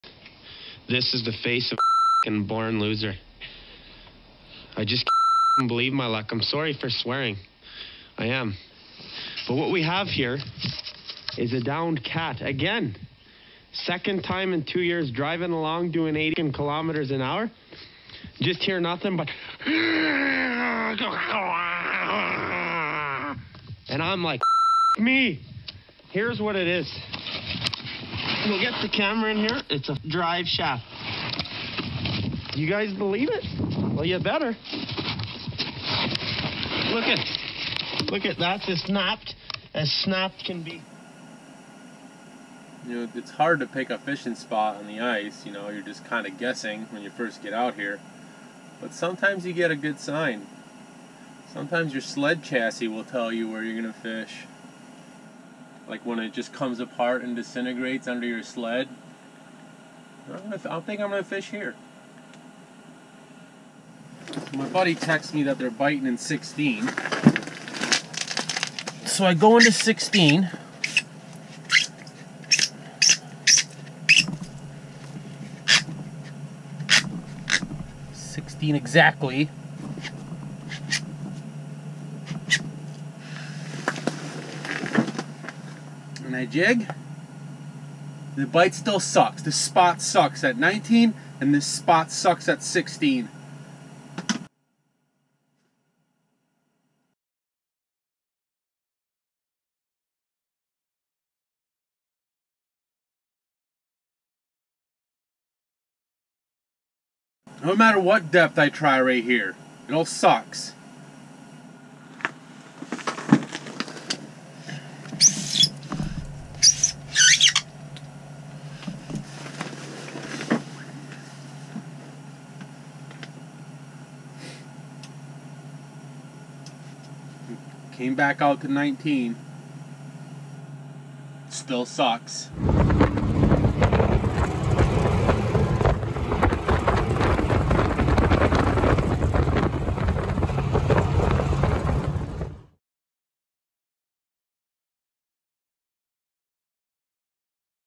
Saginaw Bay Sled Breakdown and Depth Control Tips
Ice fishing Saginaw Bay can be rough on equipment, thanks to TK & Son Ice Recovery for bailing me out.